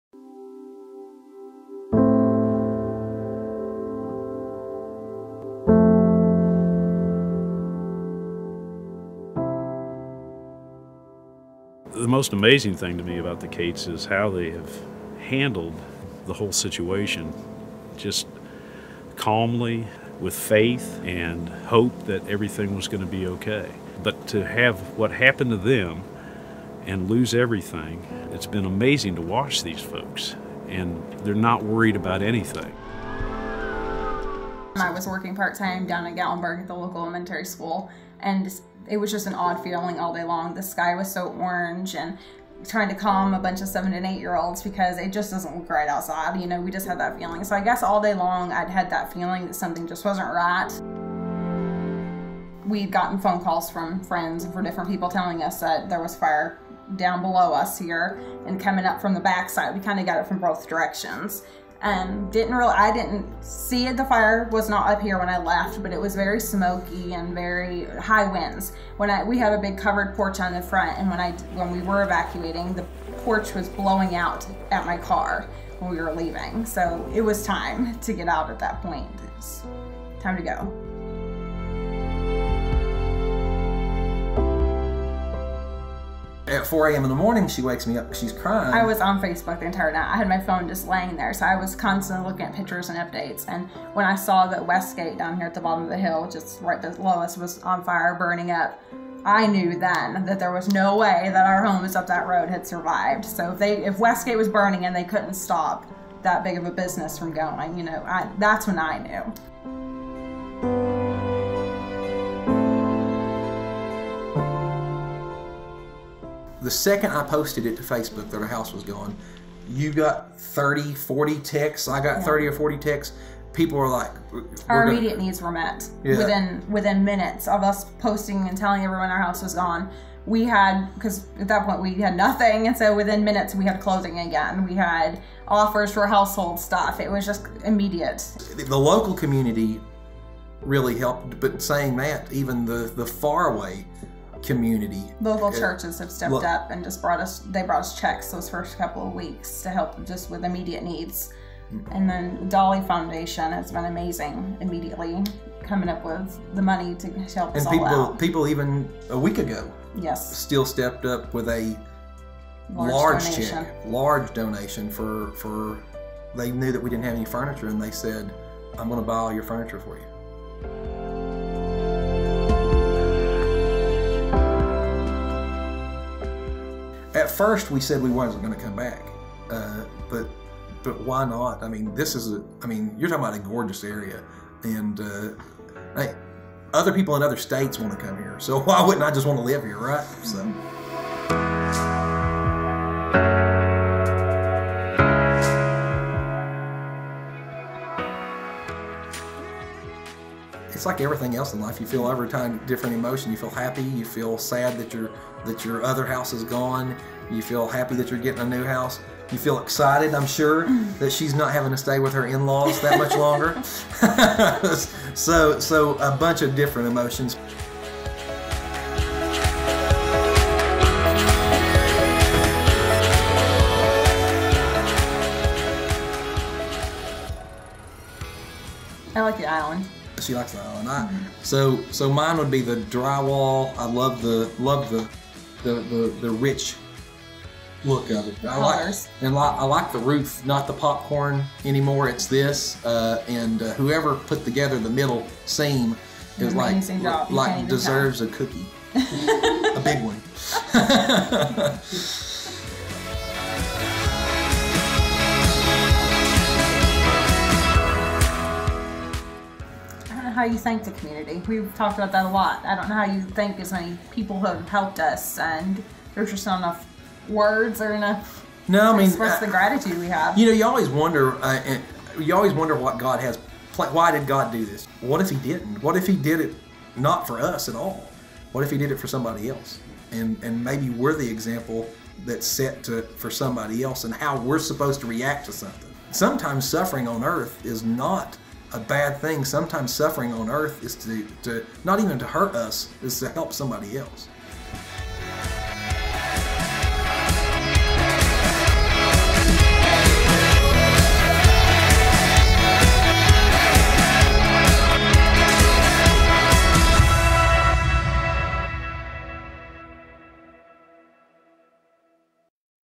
Displaced Gatlinburg Family Resettles in New Clayton Home After Wildfires
Meet the Cates family from Gatlinburg, TN who lost their home in the Gatlinburg wildfires that swept through the Smoky Mountains. The family shares their experience of evacuating Gatlinburg, how organizations and volunteers rallied to help support victims of the fires and the process for getting into a new home through a local Clayton home center.